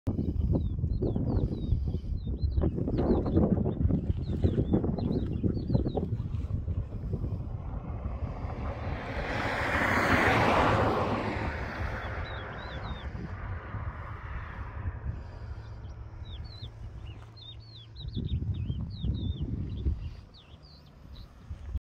Pandemic ride 2021 , From Indonesia to Kyrgiztan(3)
Songkull to Osh City